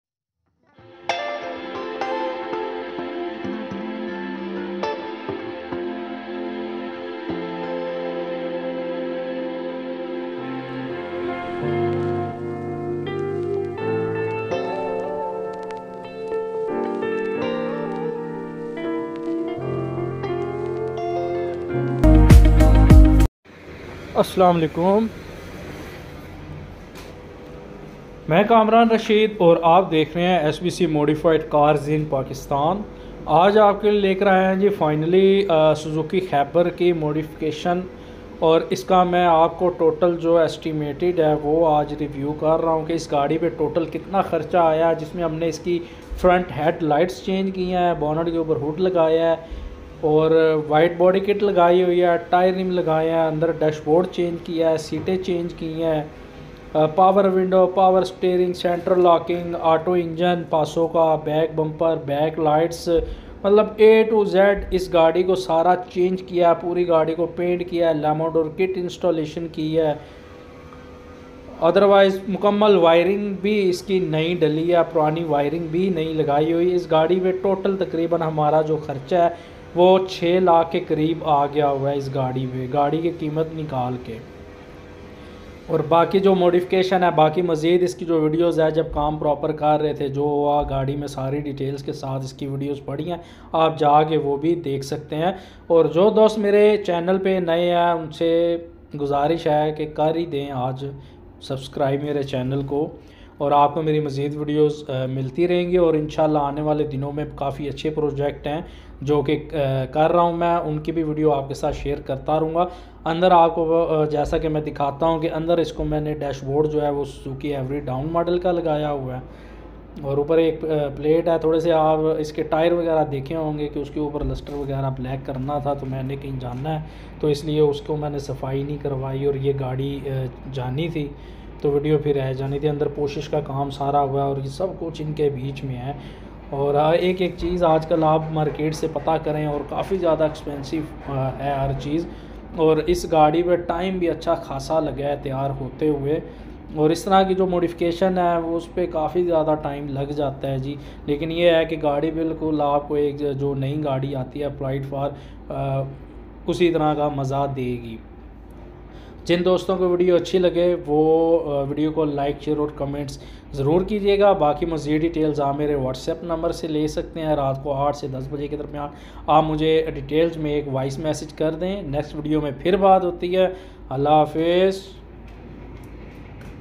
Suzuki Khyber Modification Estimate Review
Sbc Modified Cars In Pakistan

All modification cars & universal vertical lambo door kit installation all cars in pakistan

more details..

sbcmodifier.gq